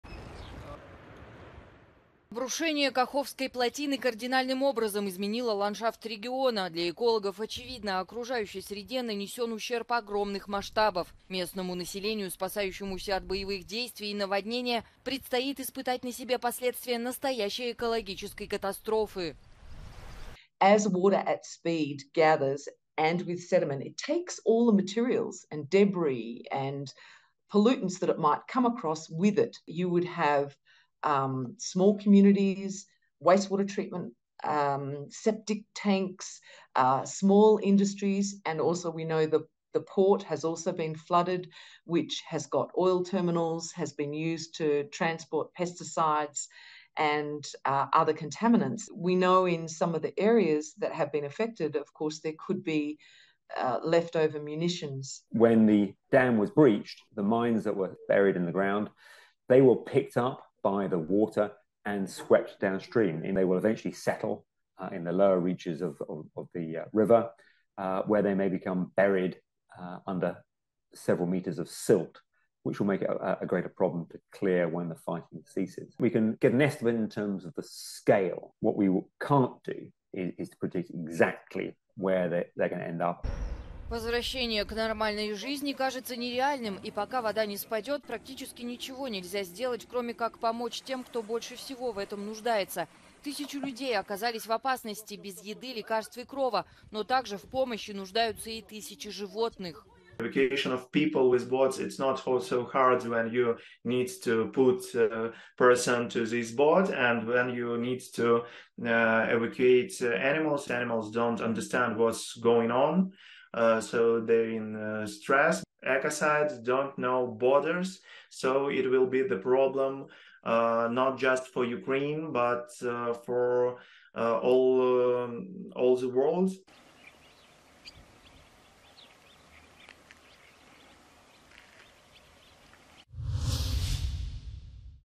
Подрыв Каховской ГЭС: последствия для экологии и риски для населения
Промышленное, биологическое загрязнение, загрязнение акватории минами и неразорвавшимися боеприпасами... Для экологов очевидно: населению, спасающемуся наводнения, предстоит испытать на себе последствия настоящей экологической катастрофы.